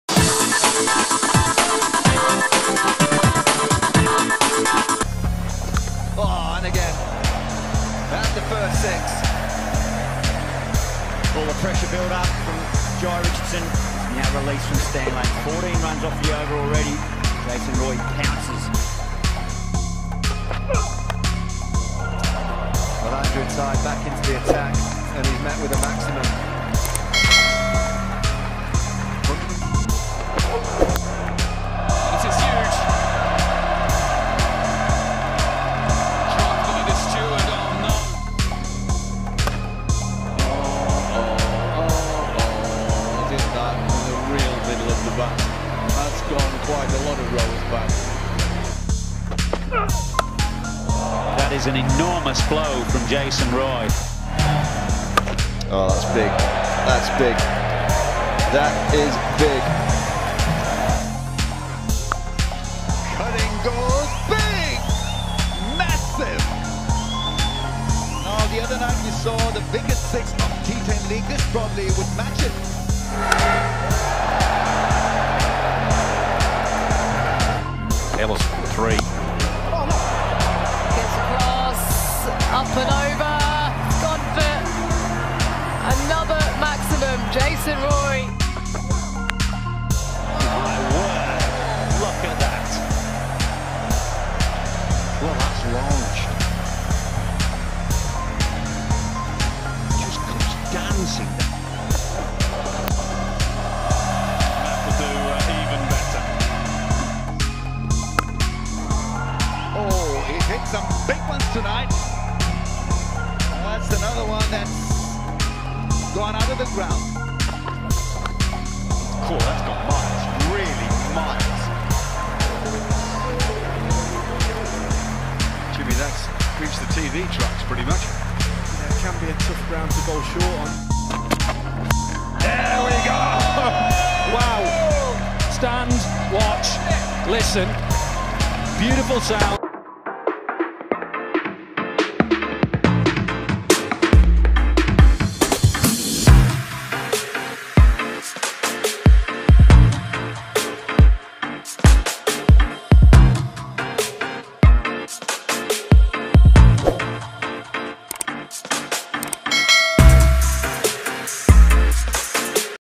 Jason Roy Top Sixes || Sixes Out of ground of Jason Roy || England Cricket
In this video we will see the FULL VIDEO of Jason Roy's top sixes.